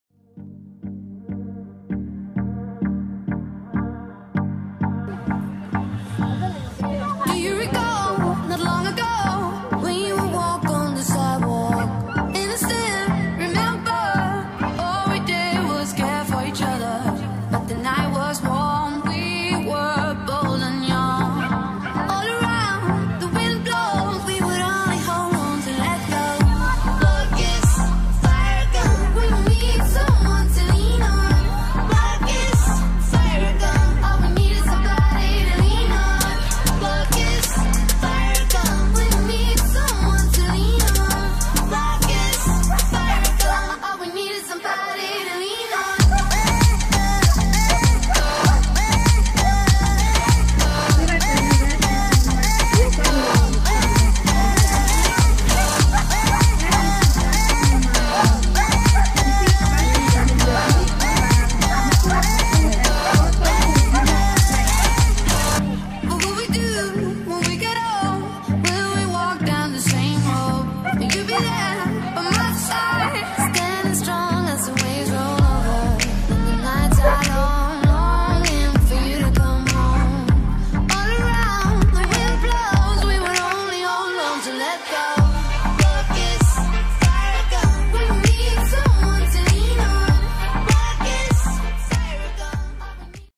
III. Veszprémvölgyi agility kupa - Tio
Harmadik szintes agility versenyünk. Tioka ismét megmutatta, hogy tud, ha akar ♥

A1 jumping: hibátlan, 2.hely
A1 agility: hibátlan, 2.hely
2015 Veszprém-völgy Agility Bajnoka ♥